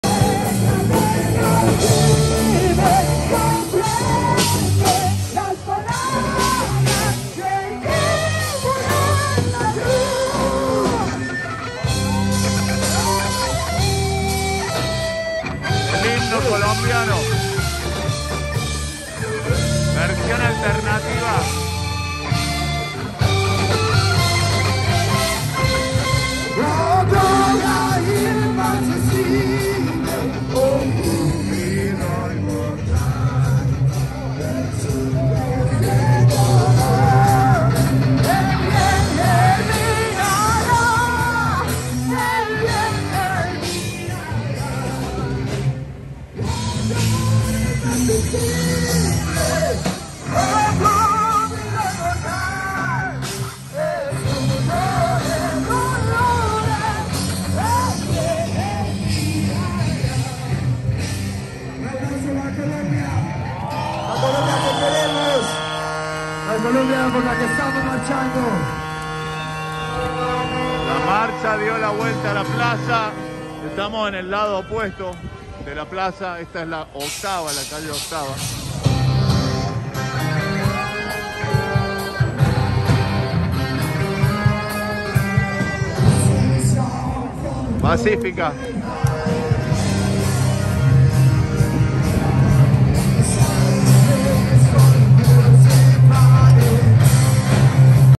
Mas de la Marcha de Protesta, Plaza Bolivar, Pereira, Colombia
Cuarta parte sobre la Carrera 8va, en el centro de la Ciudad de Pereira, Colombia. Viernes 28 de Mayo, 2021